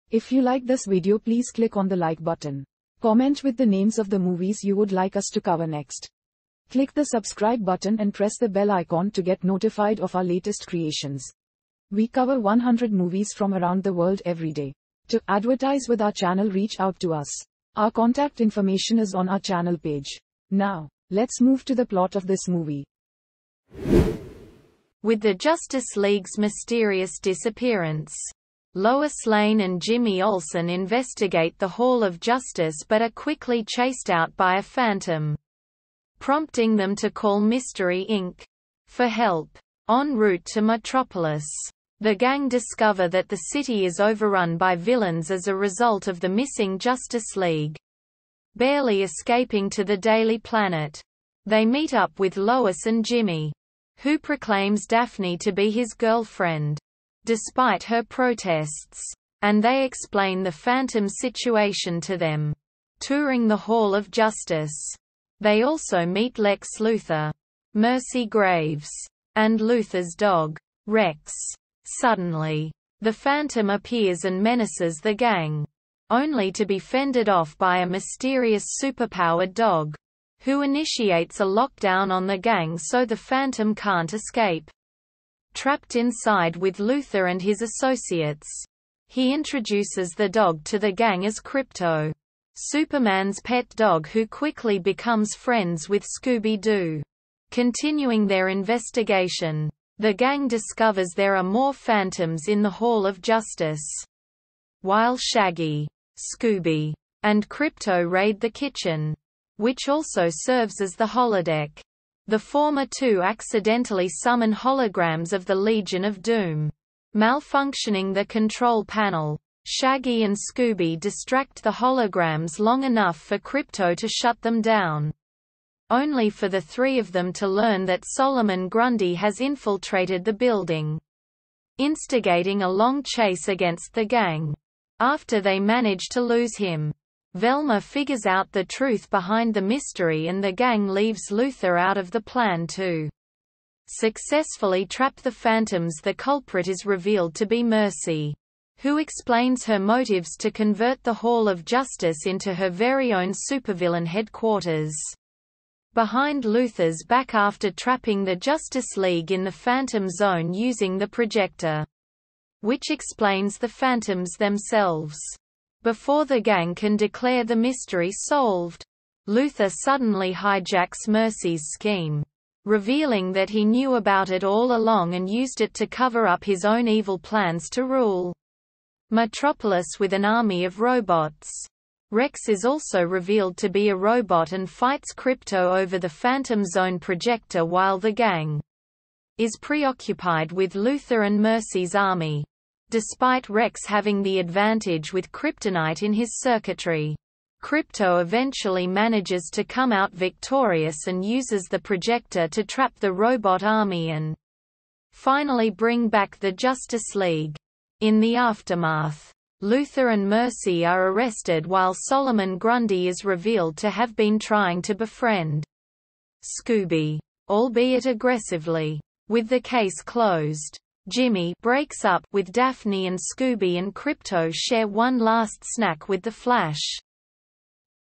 Scooby-Doo! and Krypto, Too! Plot
Scooby-Doo! and Krypto, Too! is a 2023 American animated direct-to-video mystery film produced by Warner Bros. Animation and DC Entertainment and distributed by Warner Bros. Home Entertainment. It is the thirty-eighth entry in the direct-to-video series of Scooby-Doo films.